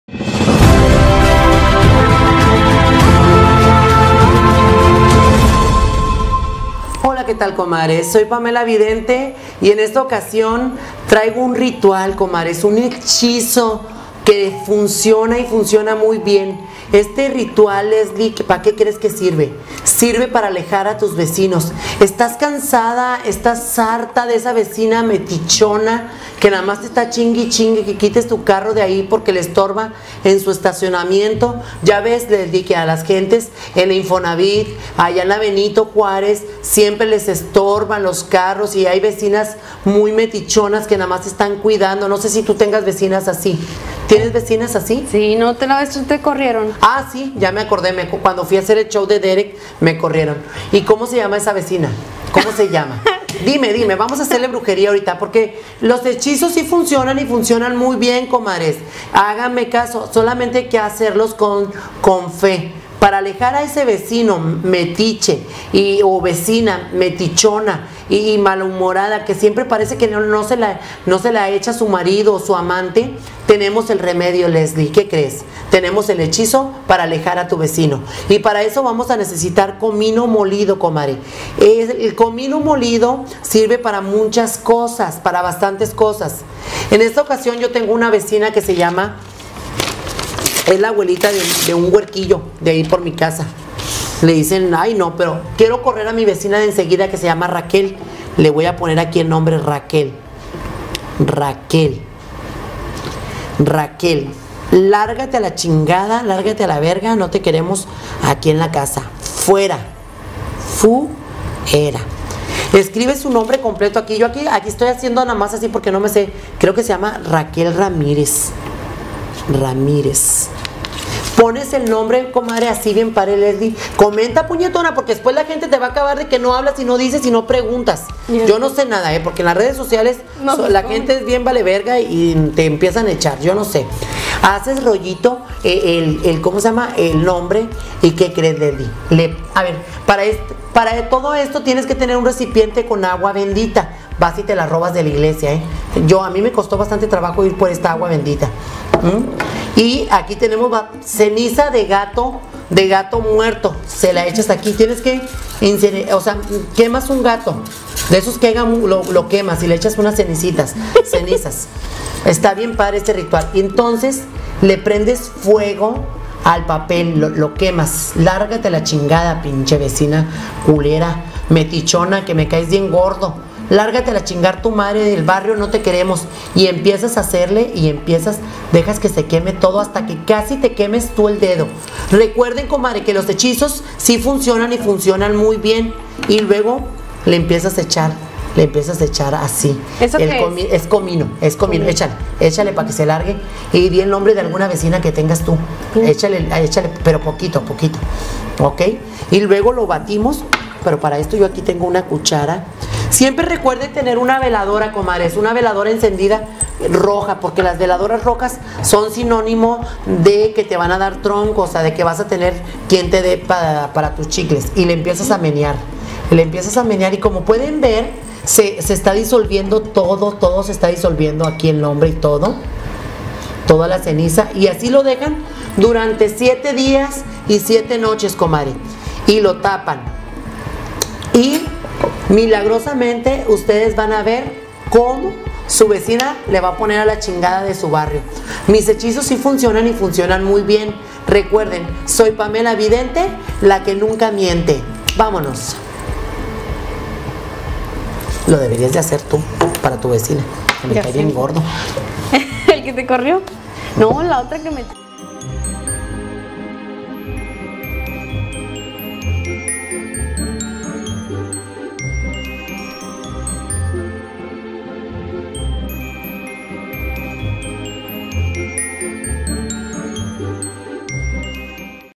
PamelaVidente el Echizo Para Alejar a tus vecinos mamones 2019
Rescate este vídeo quien no tiene una vecina chismosa que no mas esta a ver que ve  bueno yo pamelaVidente la que nunca miente y siempre anda con el  Qlo caliente les traigo  este hechizo
para alejar a tu vecina mamilas de una ves por toda fácil rápido y ensillo .
Facebook/Show Pamela Chup Payaso Prometin